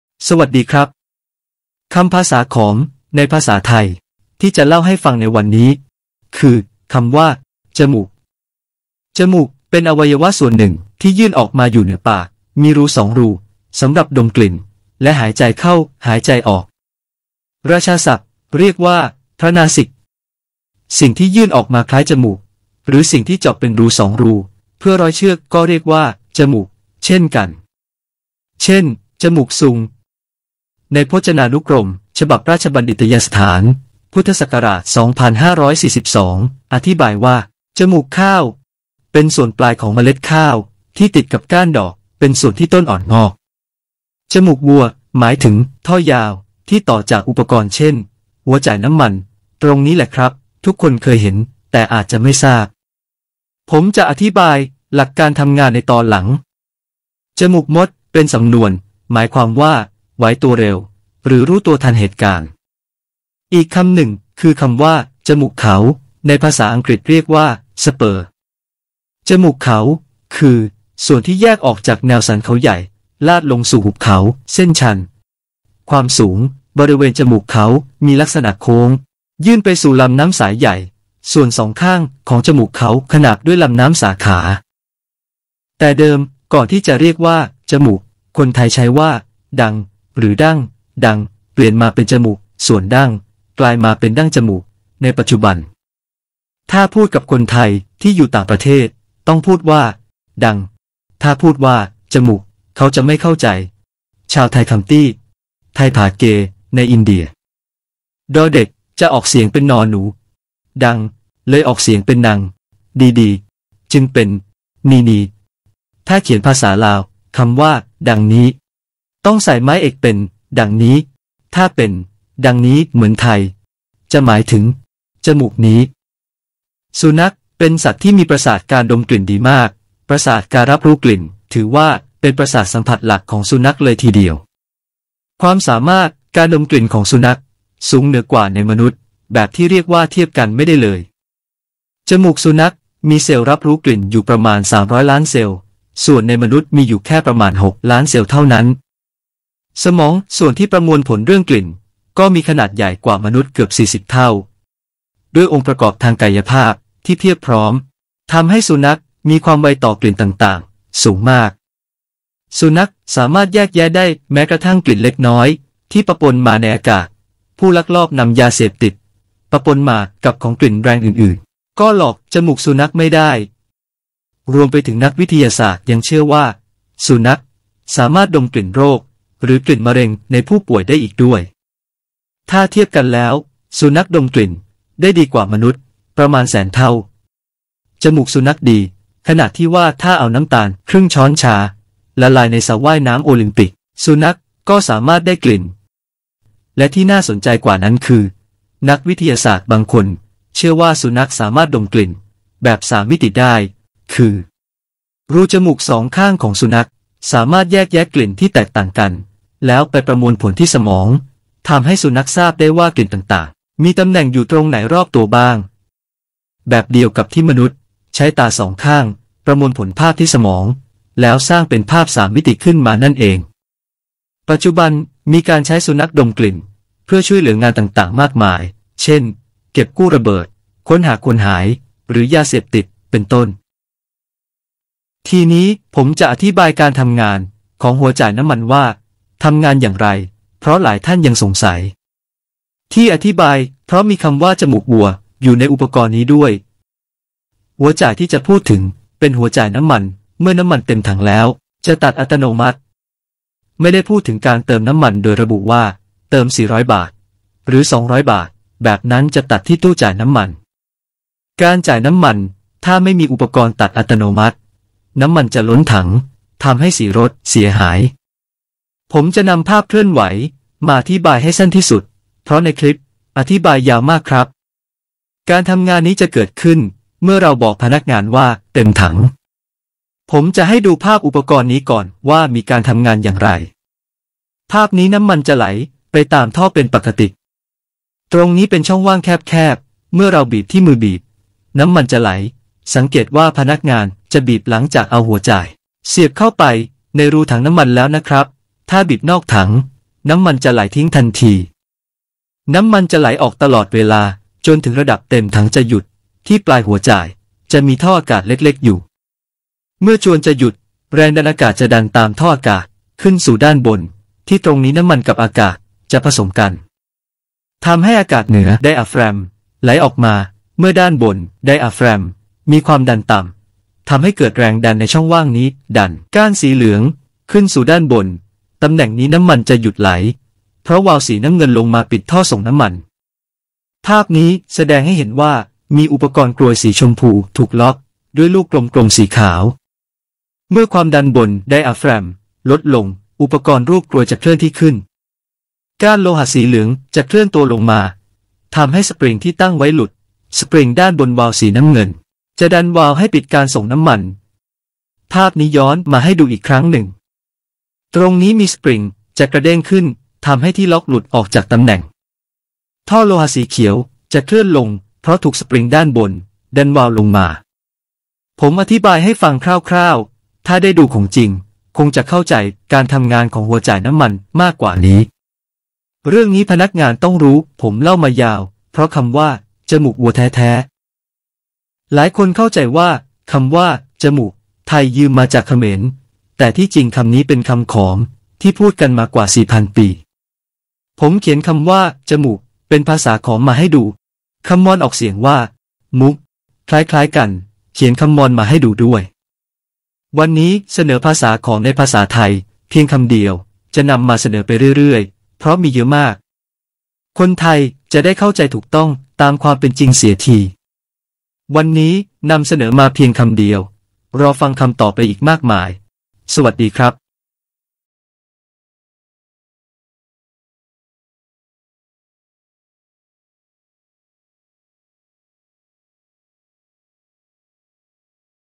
Ep271 *คำขอม ในภาษาไทย # 24 จมูก (ច្រមុះ) ภาษามอญเรียก มุห์ (မိုဟ်, မုဟ်)
สวัสดีครับ คำภาษาขอมในภาษาไทยที่จะเล่าให้ฟังในวันนี้คือ คำว่า จมูก (ច្រមុះ , .จฺรมุะ ) จมูก เป็นอวัยวะส่วนหนึ่งที่ยื่นออกมาอยู่เหนือปาก มีรู 2 รู สำหรับดมกลิ่นและหายใจเข้าหายใจออก ราชาศัพท์เรียกว่า "พระนาสิก" สิ่งที่ยื่นออกมาคล้ายจมูก หรือสิ่งที่เจาะเป็นรู 2 รูเพื่อร้อยเชือกก็เรียกว่า จมูก เช่นกัน เช่น จมูกซุง ในพจนานุกรม ฉบับราชบัณฑิตยสถาน พ.ศ. 2542 อธิบายว่า
       จมูกข้าว เป็นส่วนปลายของเมล็ดข้าวที่ติดกับก้านดอก เป็นส่วนที่ต้นอ่อนงอก จมูกวัว หมายถึง ท่อยาวที่ต่อจากอุปกรณ์เช่น หัวจ่ายน้ำมัน ตรงนี้แหละครับ ทุกคนเคยเห็นแต่อาจจะไม่ทราบ ผมจะอธิบายหลักการทำงานในตอนหลัง
        จมูกมด เป็นสำนวน หมายความว่า ไหวตัวเร็วหรือรู้ตัวทันเหตุการณ์ อีกคำหนึ่ง คือ คำว่า จมูกเขา ในภาษาอังกฤษเรียกว่า สเปอร์ (Spur) จมูกเขา คือ ส่วนที่แยกออกจากแนวสันเขาใหญ่ ลาดลงสู่หุบเขา เส้นชันความสูงบริเวณจมูกเขามีลักษณะโค้ง ยื่นไปสู่ลำน้ำสายใหญ่ ส่วนสองข้างของจมูกเขาขนาบด้วยลำน้ำสาขา แต่เดิม ก่อนที่จะเรียกว่า จมูก คนไทยใช้ว่า ดัง หรือ ดั้ง, ดัง เปลี่ยนมาเป็น จมูก ส่วนดั้ง กลายมาเป็นดั้งจมูกในปัจจุบัน ถ้าพูดกับคนไทที่อยู่ต่างประเทศ ต้องพูดว่า ดัง ถ้าพูดว่า จมูก เขาจะไม่เข้าใจ ชาวไทคำตี้ ไทผาเก ในอินเดีย ด เด็กจะออกเสียงเป็น น หนู , ดัง เลยออกเสียงเป็น นัง, ดี ดี จึงเป็น นี นี, ถ้าเขียนภาษาลาว คำว่า ดังนี้ ต้องใส่ไม้เอกเป็น ดั่งนี้ ถ้าเป็น ดังนี้ เหมือนไทย จะหมายถึง จมูกนี้
        สุนัข เป็นสัตว์ ที่มีประสาทการดมกลิ่นดีมาก ประสาทการรับรู้กลิ่นถือว่าเป็นประสาทสัมผัสหลักของสุนัขเลยทีเดียว ความสามารถการดมกลิ่นของสุนัข สูงเหนือกว่าในมนุษย์แบบที่เรียกว่าเทียบกันไม่ได้เลย จมูกสุนัขมีเซลล์รับรู้กลิ่นอยู่ประมาณ 300 ล้านเซลล์ ส่วนในมนุษย์มีอยู่แค่ประมาณ 6 ล้านเซลล์เท่านั้น สมองส่วนที่ประมวลผลเรื่องกลิ่น ก็มีขนาดใหญ่กว่ามนุษย์เกือบ 40 เท่า ด้วยองค์ประกอบทางกายภาพที่เพียบพร้อม ทำให้สุนัขมีความไวต่อกลิ่นต่างๆ สูงมาก
       สุนัขสามารถแยกแยะได้ แม้กระทั่งกลิ่นเล็กน้อยที่ปะปนมาในอากาศ ผู้ลักลอบนำยาเสพย์ติดปะปนมากับของกลิ่นแรงอื่นๆ ก็หลอกจมูกสุนัขไม่ได้ รวมไปถึงนักวิทยาศาสตร์ยังเชื่อว่า สุนัข สามารถดมกลิ่นโรคหรือกลิ่นมะเร็งในผู้ป่วยได้อีกด้วย ถ้าเทียบกันแล้ว สุนัขดมกลิ่นได้ดีกว่ามนุษย์ประมานแสนเท่า จมูกสุนัขดีขนาดที่ว่าถ้าเอาน้ำตาลครึ่งช้อนชาละลายในสระว่ายน้ำโอลิมปิก สุนัขก็สามารถได้กลิ่น และที่น่าสนใจกว่านั้นคือนักวิทยาศาสตร์บางคนเชื่อว่าสุนัขสามารถดมกลิ่นแบบสามมิติได้ คือ รูจมูกสองข้างของสุนัข สามารถแยกแยะกลิ่นที่แตกต่างกันแล้วไปประมวลผลที่สมอง ทำให้สุนัขทราบได้ว่ากลิ่นต่างๆ มีตำแหน่งอยู่ตรงไหนรอบตัวบ้าง แบบเดียวกับที่มนุษย์ใช้ตาสองข้างประมวลผลภาพที่สมองแล้วสร้างเป็นภาพสามมิติขึ้นมานั่นเอง
         ปัจจุบันมีการใช้สุนัขดมกลิ่น เพื่อช่วยเหลืองานต่างๆ มากมาย เช่น เก็บกู้ระเบิด ค้นหาคนหาย หรือยาเสพย์ติด เป็นต้น
        ทีนี้ผมจะอธิบายการทำงานของหัวจ่ายน้ำมันว่าทำงานอย่างไร เพราะหลายท่านยังสงสัย ที่อธิบายเพราะมีคำว่าจมูกวัว อยู่ในอุปกรณ์นี้ด้วย หัวจ่ายที่จะพูดถึง เป็นหัวจ่ายน้ำมันเมื่อน้ำมันเต็มถังแล้ว จะตัดอัตโนมัติ ไม่ได้พูดถึงการเติมน้ำมันโดยระบุว่า เติม 400 บาท หรือ 200 บาท แบบนั้นจะตัดที่ตู้จ่ายน้ำมัน การจ่ายน้ำมัน ถ้าไม่มีอุปกรณ์ตัดอัตโนมัติ น้ำมันจะล้นถัง ทำให้สีรถเสียหาย ผมจะนำภาพเคลื่อนไหว มาอธิบายให้สั้นที่สุด เพราะในคลิปอธิบายยาวมากครับ
        การทำงานนี้จะเกิดขึ้น เมื่อเราบอกพนักงานว่า เต็มถัง ผมจะให้ดูภาพอุปกรณ์นี้ก่อนว่ามีการทำงานอย่างไร  ภาพนี้น้ำมันจะไหลไปตามท่อเป็นปกติ ตรงนี้เป็นช่องว่างแคบ ๆ เมื่อเราบีบที่มือบีบ น้ำมันจะไหล สังเกตว่าพนักงานจะบีบหลังจากเอาหัวจ่าย เสียบเข้าไปในรูถังน้ำมันแล้วนะครับ ถ้าบีบนอกถังน้ำมันจะไหลทิ้งทันที
น้ำมันจะไหลออกตลอดเวลา จนถึงระดับเต็มถังจะหยุด ที่ปลายหัวจ่ายจะมีท่ออากาศเล็กๆ อยู่ เมื่อจวนจะหยุด แรงดันอากาศจะดันตามท่ออากาศ ขึ้นสู่ด้านบน ที่ตรงนี้น้ำมันกับอากาศจะผสมกัน ทำให้อากาศเหนือไดอะแฟรมไหลออกมา เมื่อด้านบนไดอะแฟรมมีความดันต่ำ ทำให้เกิดแรงดันในช่องว่างนี้ ดัน ก้านสีเหลือง ขึ้นสู่ด้านบน ตำแหน่งนี้น้ำมันจะหยุดไหล เพราะวาล์วสีน้ำเงินลงมาปิดท่อส่งน้ำมัน
        ภาพนี้แสดงให้เห็นว่ามีอุปกรณ์กรวยสีชมพูถูกล็อกด้วยลูกกลม ๆ สีขาว เมื่อความดันบนไดอะแฟรมลดลง อุปกรณ์รูปกรวยจะเคลื่อนที่ขึ้น ก้านโลหะสีเหลืองจะเคลื่อนตัวลงมา ทำให้สปริงที่ตั้งไว้หลุด สปริงด้านบนวาล์วสีน้ำเงิน จะดันวาวให้ปิดการส่งน้ำมัน ภาพนี้ย้อนมาให้ดูอีกครั้งหนึ่ง ตรงนี้มีสปริง จะกระเด้งขึ้นทำให้ที่ล็อกหลุดออกจากตำแหน่ง ท่อโลหะสีเขียวจะเคลื่อนลงเพราะถูกสปริงด้านบนดันวาล์วลงมา ผมอธิบายให้ฟังคร่าว ๆ ถ้าได้ดูของจริง คงจะเข้าใจการทำงานของหัวจ่ายน้ำมันมากกว่านี้ เรื่องนี้พนักงานต้องรู้ ผมเล่ามายาวเพราะคำว่าจมูกวัวแท้ ๆ
         หลายคนเข้าใจว่า คำว่า จมูก ไทยยืมมาจากเขมร แต่ที่จริงคำนี้เป็นคำขอม ที่พูดกันมา กว่า 4,000 ปี
ผมเขียนคำว่า จมูก เป็นภาษาขอมมาให้ดู คำมอญออกเสียงว่า มุฮ์  คล้าย ๆ กัน เขียนคำมอญมาให้ดูด้วย
          วันนี้เสนอภาษาขอมในภาษาไทยเพียงคำเดียว จะนำมาเสนอไปเรื่อย ๆ เพราะมีเยอะมาก คนไทยจะได้เข้าใจถูกต้อง ตามความเป็นจริงเสียที
          วันนี้ นำเสนอมาเพียงคำเดียว รอฟังคำต่อไปอีกมากมาย สวัสดีครับ